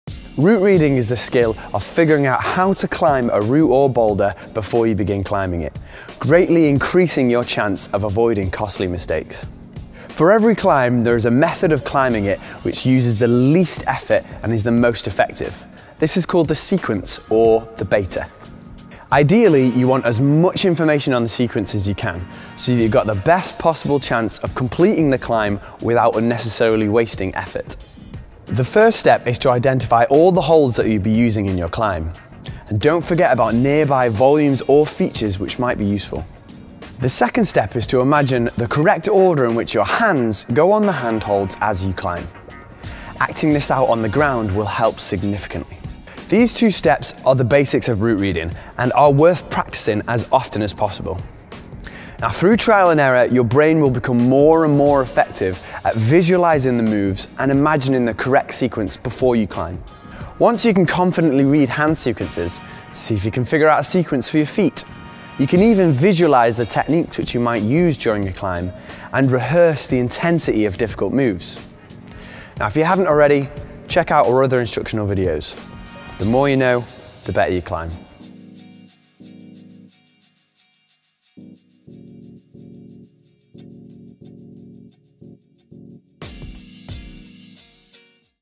How to route read a climb or boulder
Before going on a road trip you plan your journey on a map* to work out the quickest and easiest way to your destination.
Route reading is you planning the easiest way up the climb before setting off. Get it right and you've a much greater chance of reaching your destination.
*Very few people plan their car journeys now due to SatNav! Until someone develops a SatNav for climbs, learn to route read!